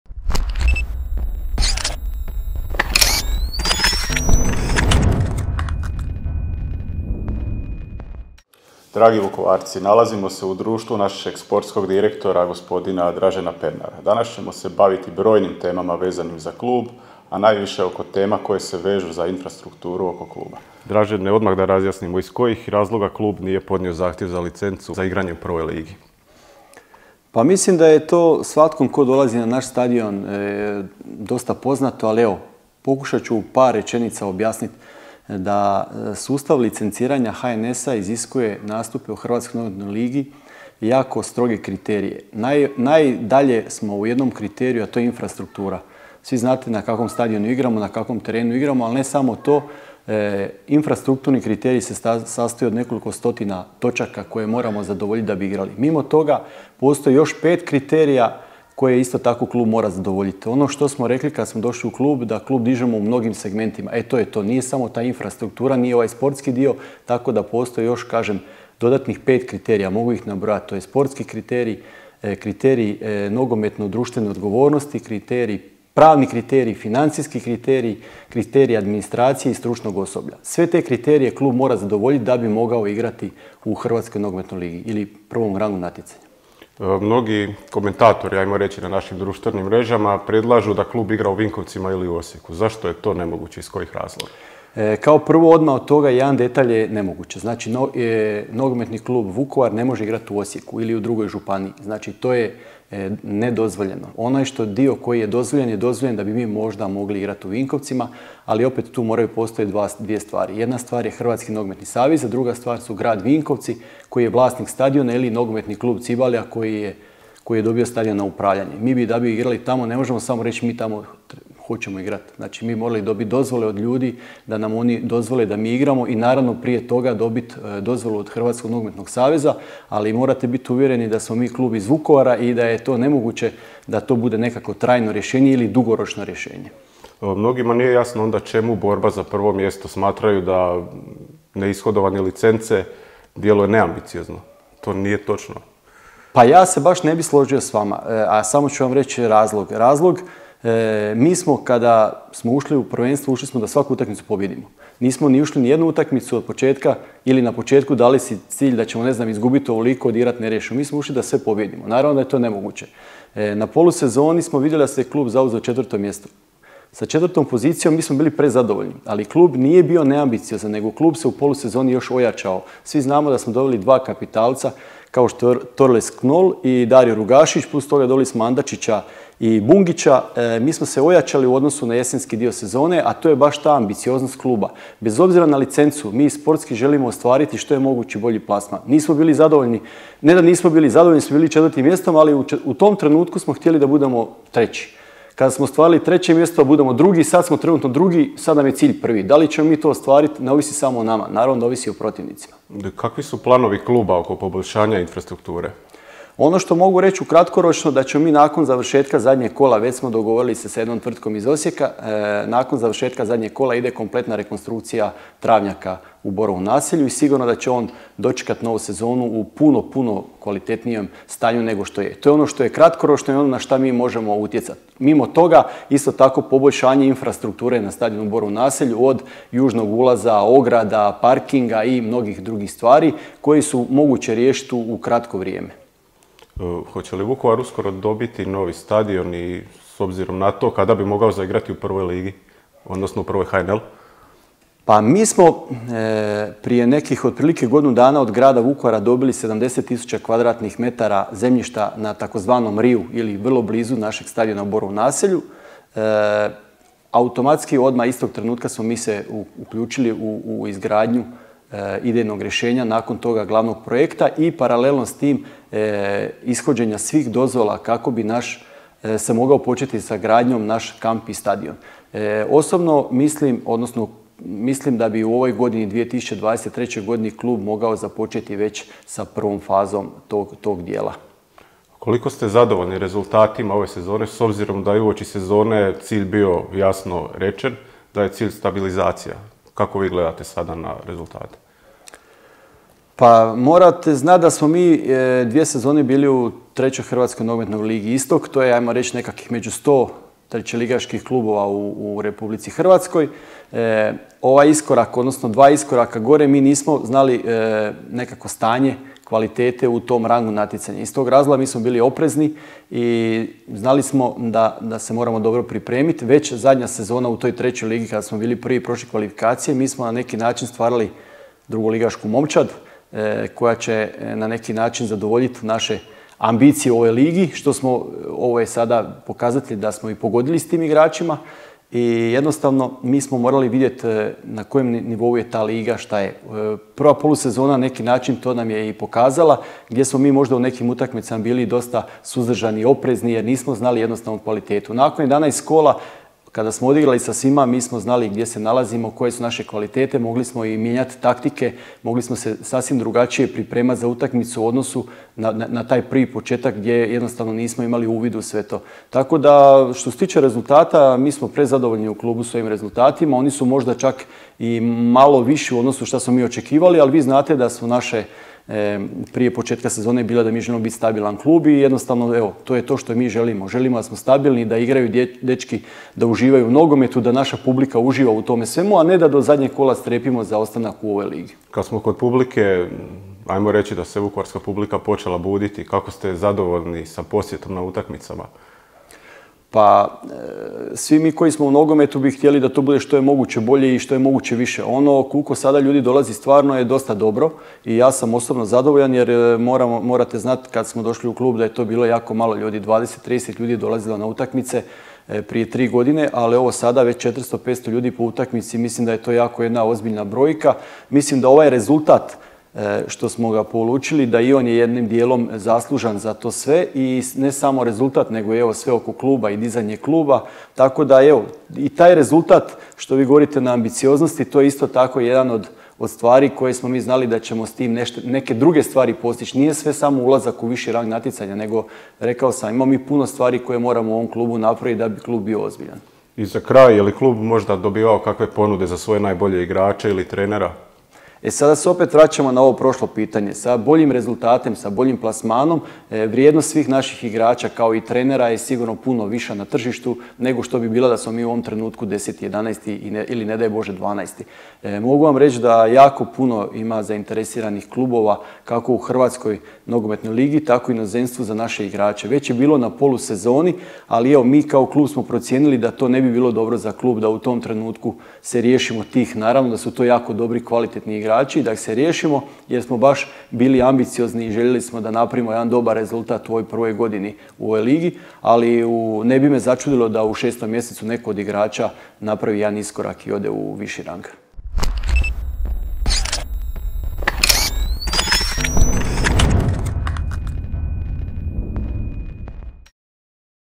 Razgovarali smo s našim sportskim direktorom Draženom Pernarom
Razgovarali smo s našim sportskim direktorom Draženom Pernarom, koji nam je stručno objasnio kompletnu situaciju oko svih gorućih klupskih tema.

❌ zašto Vukovar (još) ne može igrati u HNL-u
❓ pitanje klupskih ambicija i infrastrukture
👉 kada bi Vukovar mogao dobiti svoj stadion i sportski kompleks

... i ne samo to. Saznajte odgovore na pitanja koja su vas najviše zanimala